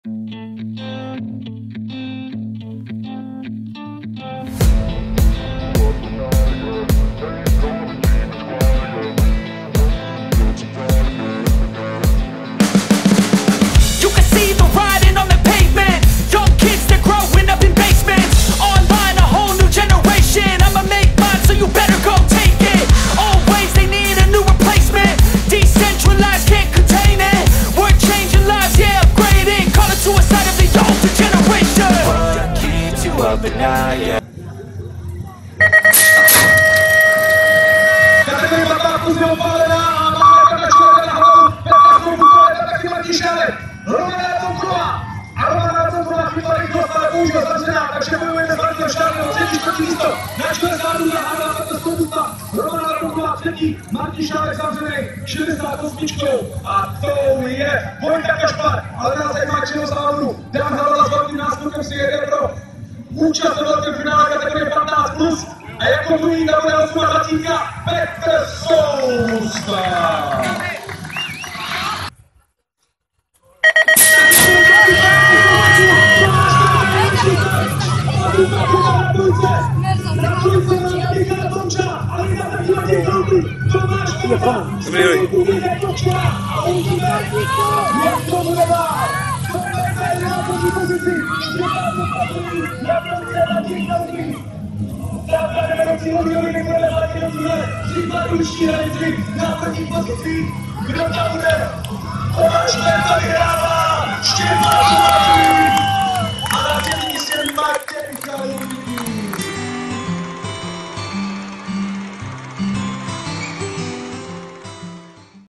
1. Český pohár 2016 - Pardubice
3.4.2016
BMX Pardubice
B15/16 1.Romana Labounková 2.Vojtěch Kašpar 3.Martin Šálek
B17+ 1.Tomáš Merta  2.Štěpán Tumpach 3.Matěj Chalupný